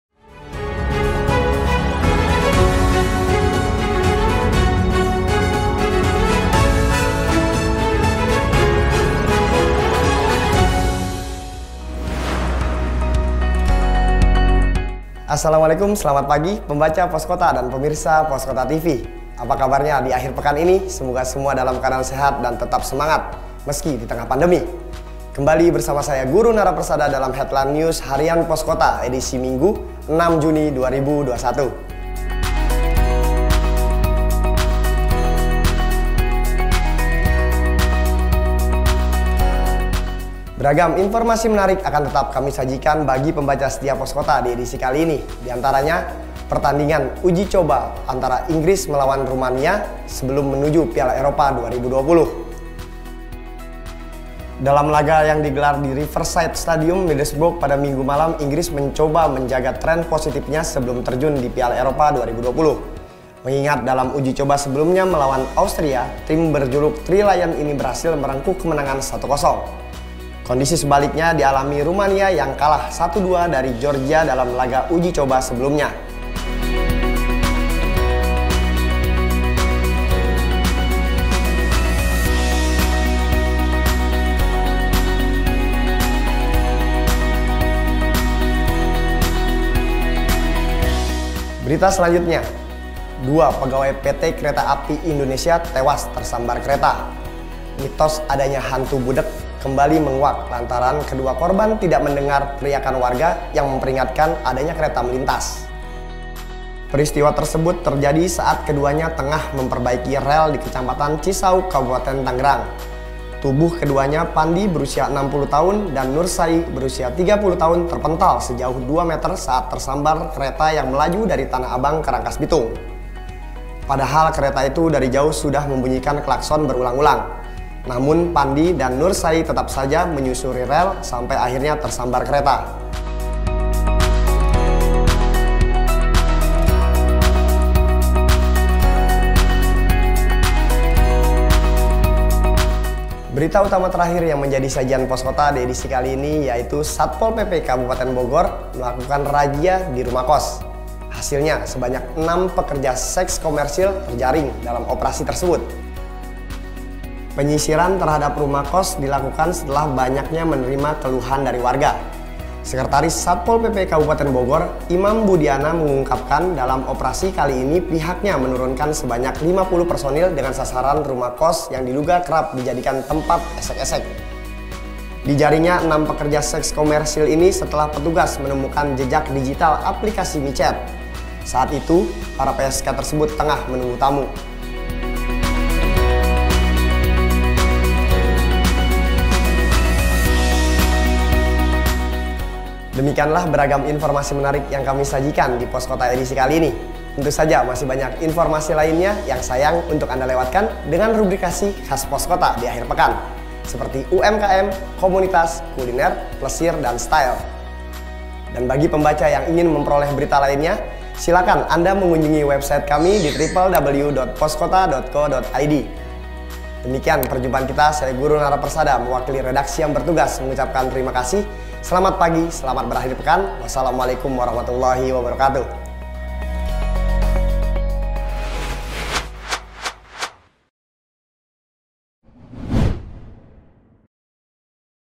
HEADLINE HARIAN POSKOTA EDISI MINGGU 6 JUNI 2021
Harian Poskota menyajikan beberapa topik menarik di antaranya preview pertandingan uji coba Inggris vs Rumania, mitos Hantu Budeg menguak setelah dua pegawai PT KAI Tewas tersambar kereta, 6 PSK di Bogor terjaring razia rumah kos.

Juga berita-berita rubrikasi khas Poskota akhir pekan seperti UMKM, Kuliner, Komunitas, Plesir dan Style.